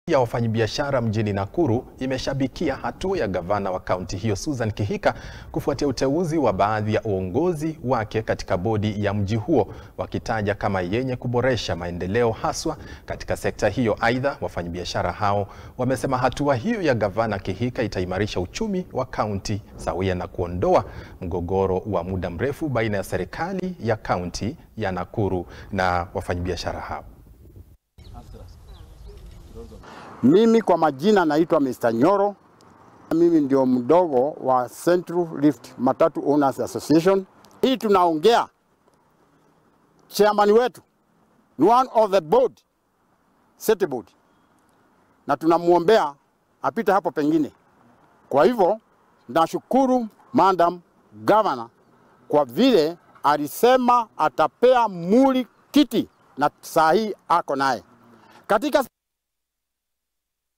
Jamii Ya Wafanyibiashara Mjini Nakuru Imeshabikia Hatua Ya Gavana Wa Kaunti Hiyo Susan Kihika
Jamii Ya Wafanyibiashara Mjini Nakuru Imeshabikia Hatua Ya Gavana Wa Kaunti Hiyo Susan Kihika Kufuatia Uteuzi Wa  Baadhi Ya Uongozi Wake Katika Bodi Ya Mji Huo, Wakitaja Kama Yenye Kuboresha Maendeleo Haswa Katika Sekta Hiyo.Aidha Wafanyibiashara Hao Wamesema Hatua Hiyo Ya Gavana Kihika  Itaimarisha Uchumi Wa Kaunti Sawia Na Kuondoa Mgogoro Wa Muda Mrefu Baina Ya Serikali Ya Kaunti Ya Nakuru Na Muungano Wa Wamiliki Wa Matatu (MOA)